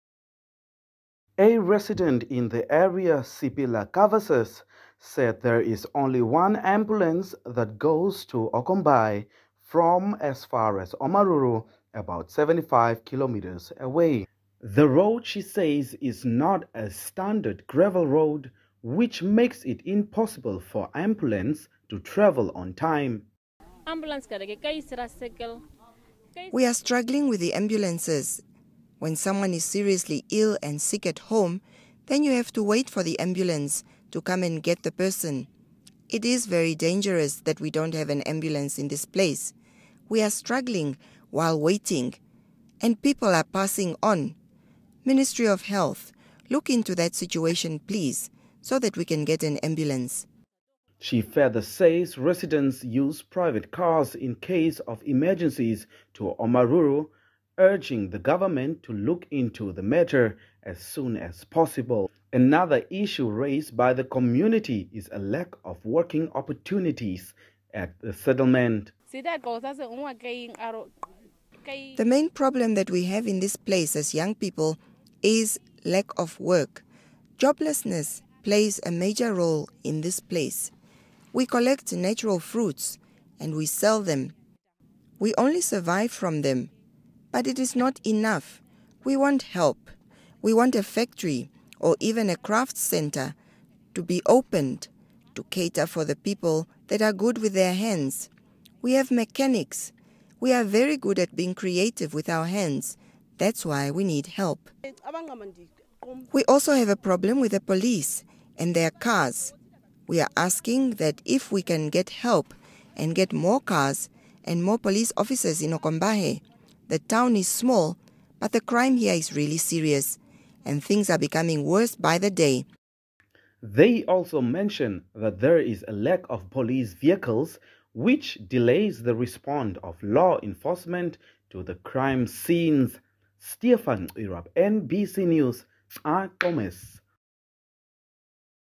Lack of ambulances hamper access to maternal health service at Okombahe settlement- nbc
Pregnant women at Okombahe settlement in Erongo's Daures Constituency are struggling to access maternal health services due to a lack of ambulances.

A resident in the area, Sibilla Gaweses, said there is only one ambulance that goes to Okombahe from as far away as Omaruru, about 75 kilometers away.

The road, she says, is not a standard gravel road, which makes it impossible for ambulances to arrive on time.